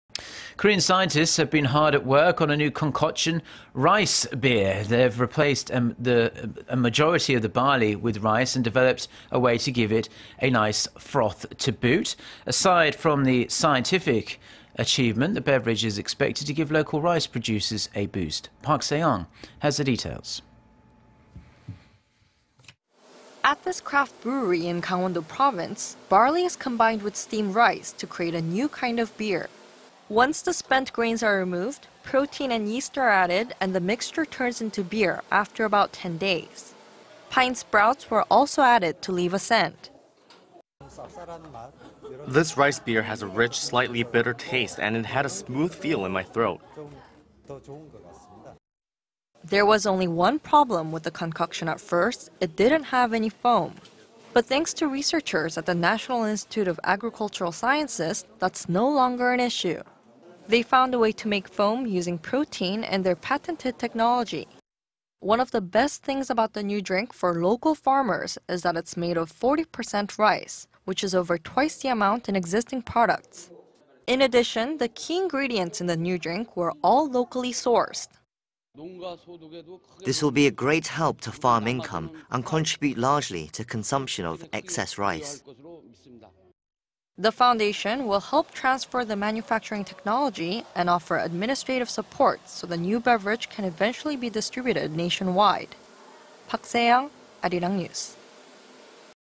Korean researchers develop rice beer, giving local farmers a boost
맥주의 이색 변신...쌀로 만든 '한국형 맥주'
Korean scientists have been hard at work on a new concoction -- rice beer.
They've replaced a majority of the barley with rice and developed a way to give it a nice froth to boot.
Aside from the scientific achievement, the beverage is expected to give local rice producers a boost.
Park Se-young has the details.

At this craft brewery in Gangwon-do Province, barley is combined with steamed rice to create a new kind of beer.
Once the spent grains are removed, protein and yeast are added and the mixture turns into beer after about ten days.
Pine sprouts were also added to leave a scent.

"This rice beer has a rich, slightly bitter taste and it had a smooth feel in my throat."

There was only one problem with the concoction at first -- it didn't have any foam.
But thanks to researchers at the National Institute of Agricultural Sciences, that's no longer an issue.
They found a way to make foam using protein and their patented technology.

One of the best things about the new drink for local farmers... is that it is made of 40 percent rice, which is over twice the amount in existing products.
In addition, the key ingredients in the new drink were all locally sourced.

"This will be a great help to farm income and contribute largely to consumption of excess rice."

The foundation will help transfer the manufacturing technology and offer administrative support so the new beverage can eventually be distributed nationwide.